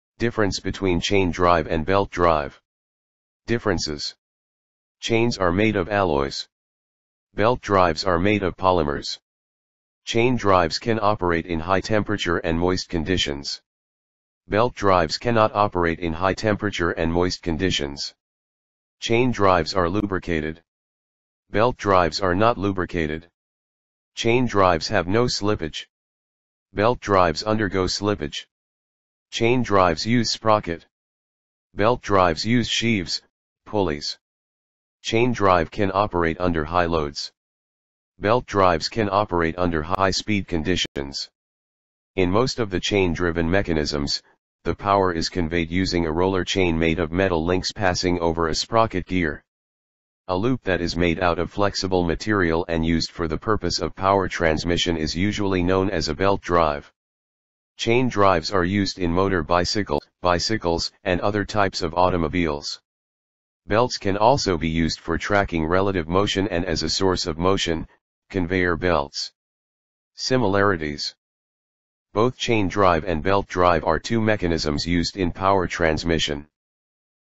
Differences of Chain Drive and Belt Drive
Both chain drives and belt drives are methods to transfer power and objects within a machine. The main difference between the two drives is that a chain drive is powered by a chain loop, while a belt drive is powered by a belt. 2.Another notable difference is the material of the drive.Belt Drive Garage Door Openers. Most people feel that the belt drive is the opener that is best. These drives openers operate on a rubber belts that are just as reliable as the chain drives. ... On the other hand, belt drives will cost a little more than a chain drive.Belt drive openers run on a rubber belt and are as reliable as chain drives. But they are virtually silent! So if your garage is attached to the house, and you hate the noise involved with garage doors, this is definitely a good choice. However, belt drives cost more then chain drives.Highly efficient, chain drives gives the advantage of more power compared to belts. It can be used for both small and large center distances. Chain drives have low maintenance cost. They give a high transmission efficiency of up to 98 percent.